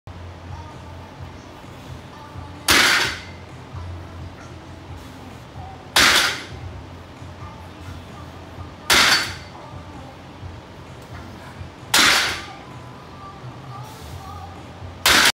Banded Rack Pulls
I did this fun drop set the other day where I started as shown here, standing on a 9" riser. I did 5 reps, then rested 30-45 seconds and did another 5 reps on a 8" riser.
I decreased the riser height 1" at a time & knocked out sets of 5 with the same rest scheme, until I was standing on that 4" aerobic step at the bottom.
Used 115 lbs and elitefts pro short mini bands here!